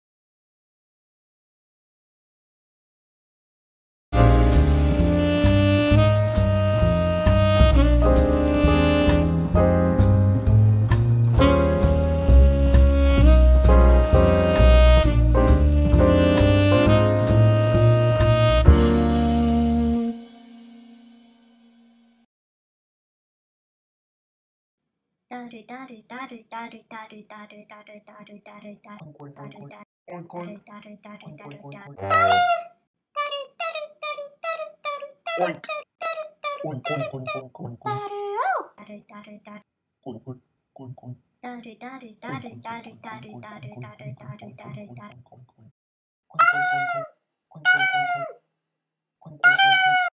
knitbits Shorts: Daru Daru a Story of friendship (Amigurumi Stop Motion Animation)
What would you like to see these two friends do next?

Daru Daru is a slow turtle. While Piggy Piggy is a fast runner. Watch as these two become good friends.

Josephine Ruff-Sloan
Artist, Animator, and Art Fairy Godmother